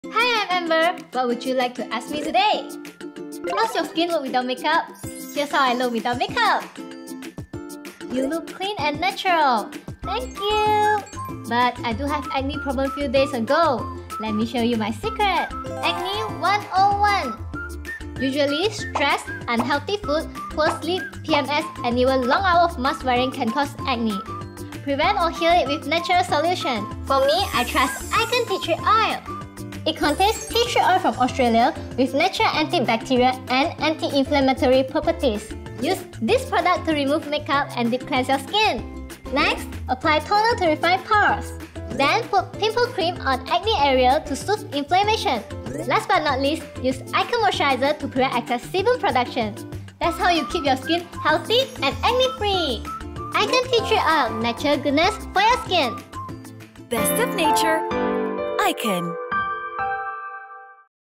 AikenTeaTreeOil 4 Simple Steps for Healthy, Acne-Free Skin
Aiken oil & acne control skincare range is formulated with Aiken Tea Tree Oil (Melaleuca Alternifolia) from Australia to help protect skin from pimples naturally. Aiken Tea Tree Oil products are suitable for oily and acne-prone skin.